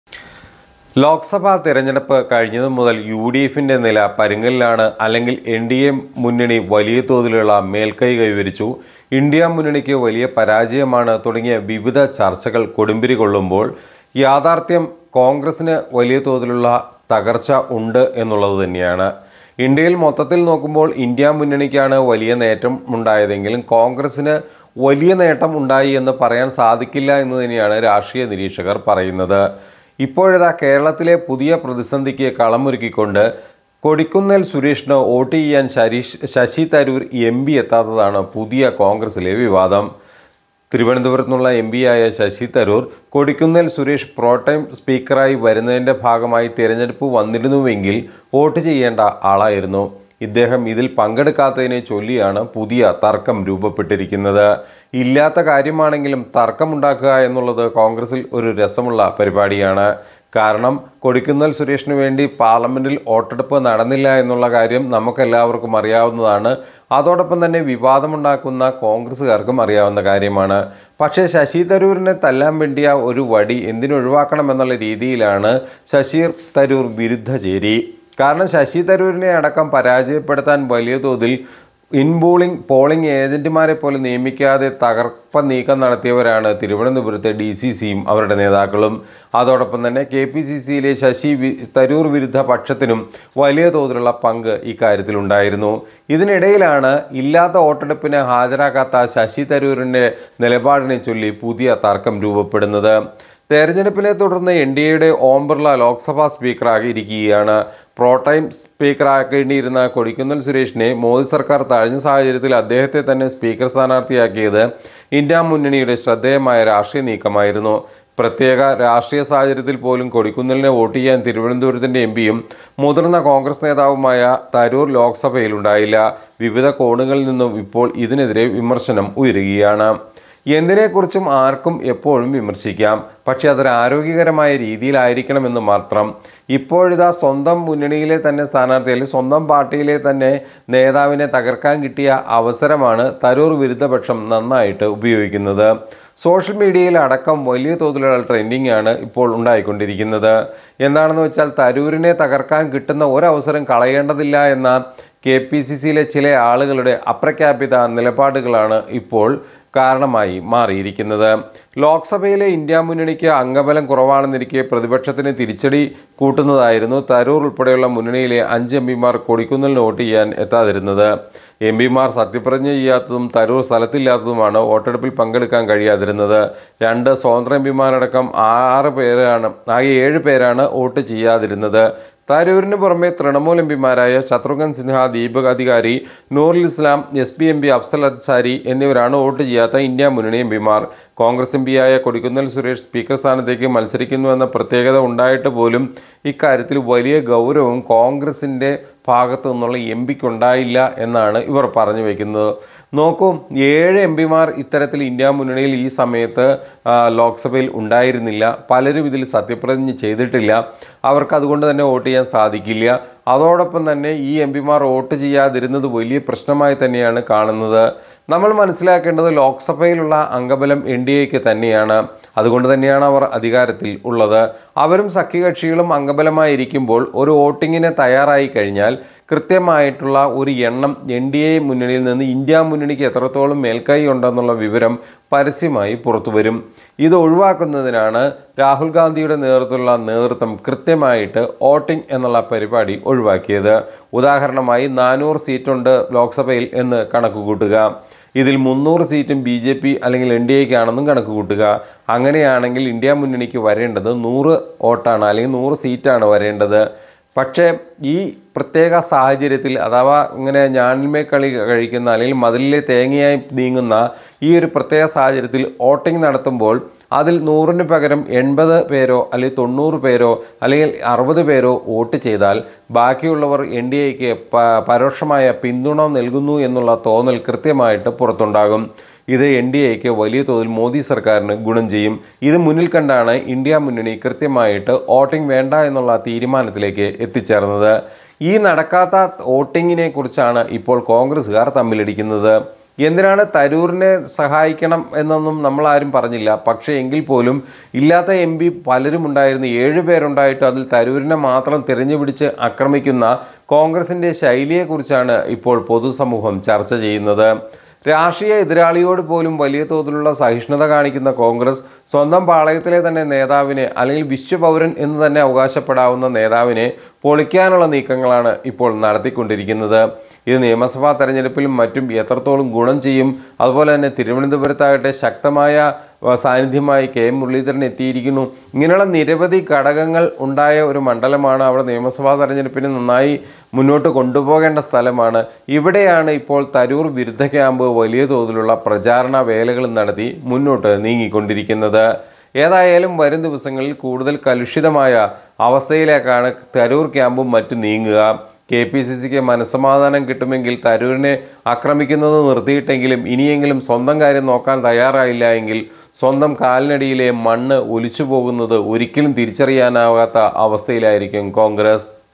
SASI THAROOR | എംപിക്കെതിരെ വിമര്‍ശനവുമായി കോണ്‍ഗ്രസ് സൈബര്‍ കിളികള്‍|CONGRESS|UDF|CPM|LDF|BJP|CPI
കൊടിക്കുന്നിലിന് വോട്ടുചെയ്യാന്‍ തരൂര്‍ എത്തിയില്ല ; എംപിക്കെതിരെ വിമര്‍ശനവുമായി കോണ്‍ഗ്രസ് സൈബര്‍ കിളികള്‍TrendingNews